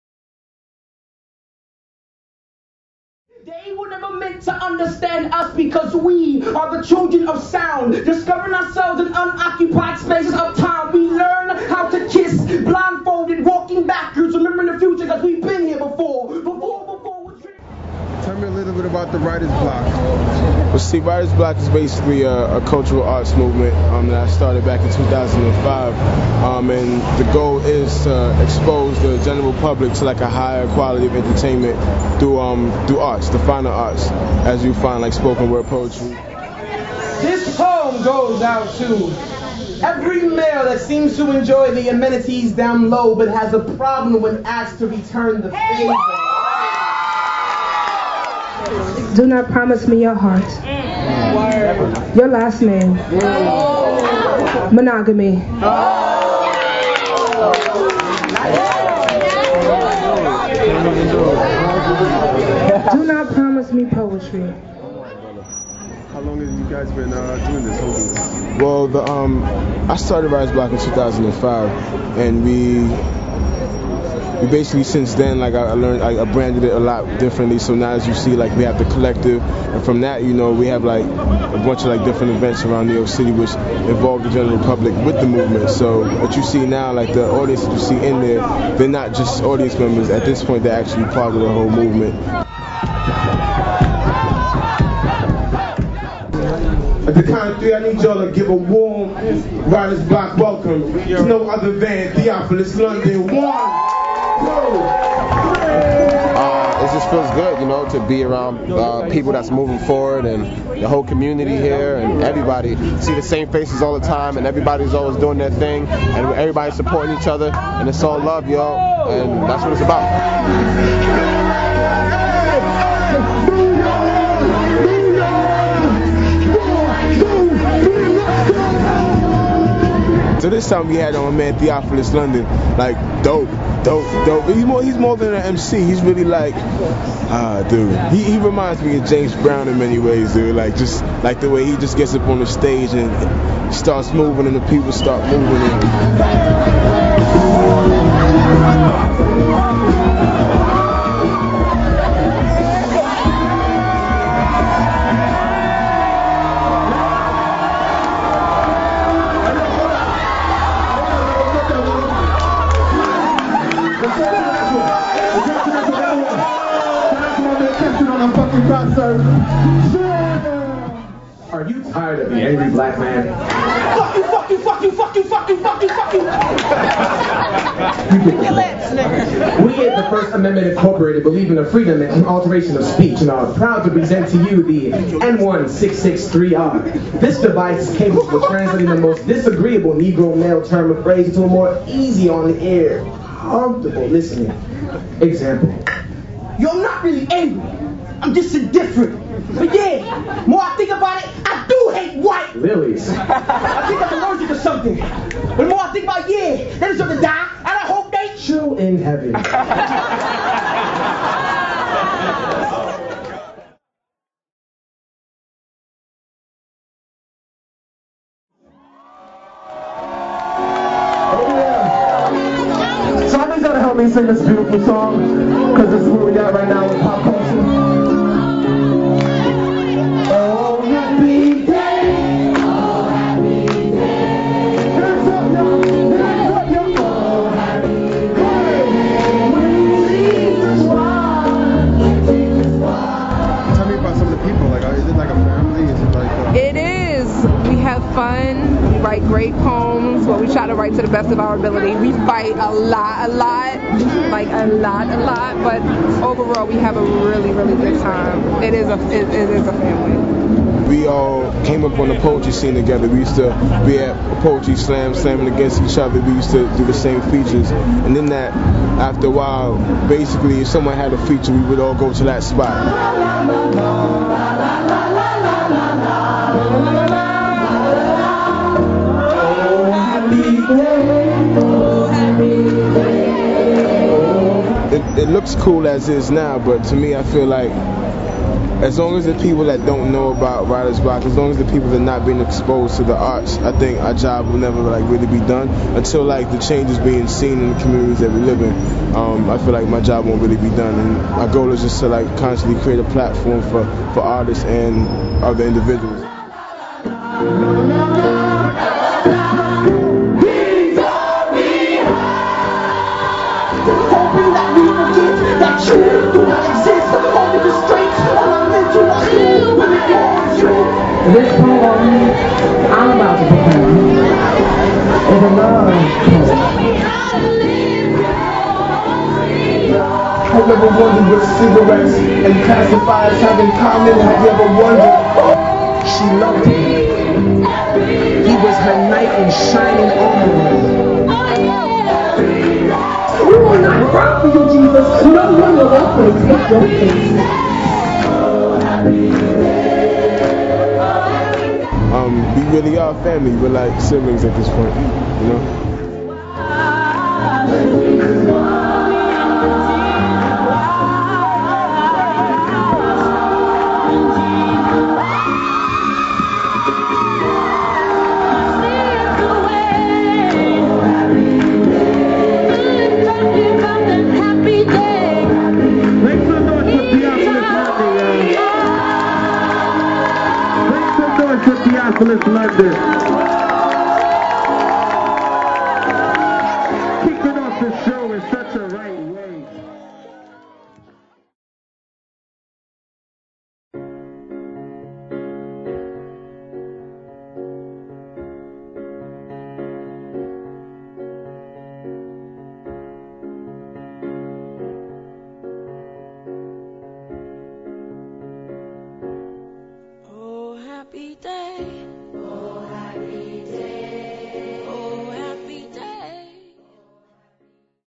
Theophilus London '08 - New Pop Vintage 2008 - Writers Block
This piece documents The Writers Block spoken word artist collective.  Shot one night at the Bowery Poets club it features founder RIP MC & Theophilus London in one of the best live performances this side of the net.